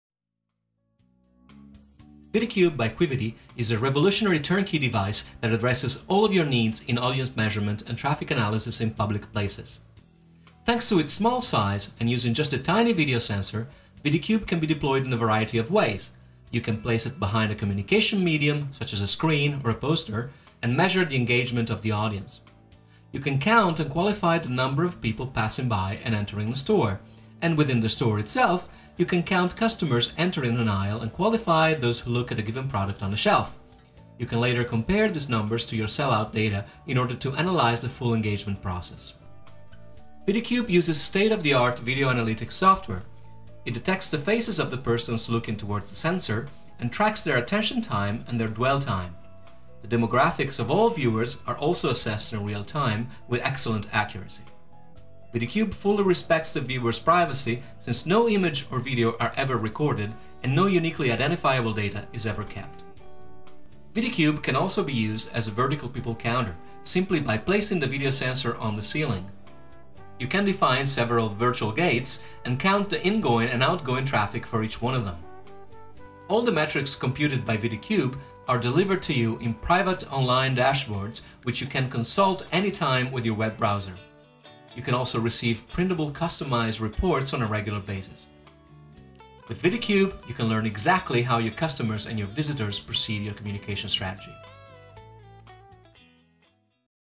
An introduction to VidiCube
The VidiCube from Quividi is a fantastic turnkey device that addresses all of your needs in audience measurement and traffic analysis in public places.
"Thanks to its diminutive size and tiny video sensor, you can use it in a large number of situations:
- you can place it behind a media, such as a screen, a poster, and assess their audience
- you can count and qualify the outside passers-by and visitors of a store
- within the store itself, you can count shoppers entering a category and taking a look to products on a shelf, and later confront this data to sell out data, in order to analyse the engagement process"
"To perform this measurement, VidiCube uses state of the art video analytics software:  it detects the faces of the persons looking towards the sensor and tracks their attention time and presence time. The demographics of all viewers is also assessed in real time with a good accuracy.
By doing so, VidiCube fully respects the viewers' privacy, since no image, no video are ever recorded, and no uniquely identifiable data is ever kept. "
Alternatively, by setting up the sensor on the ceiling, you can count the number of people crossing virtual lines, which gives you precious indications on the incoming and outcoming traffic of the observed zone
All the results are delivered to you in private online dashboards which you can consult anytime or in customized reports published on a regular basis
With VidiCube, start knowing how your visitors and shoppers perceive your communication!